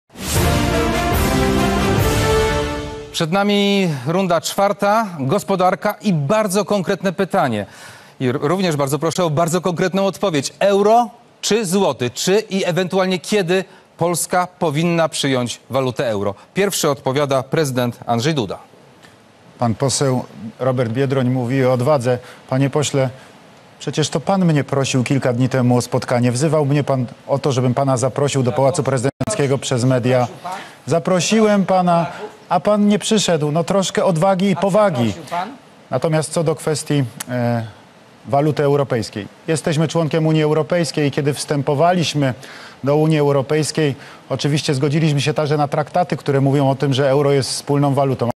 Debata Prezydencka TVP - Andrzej Duda BEZCZELNE ATAKUJE Biedronia. Nie przeprosił jego matki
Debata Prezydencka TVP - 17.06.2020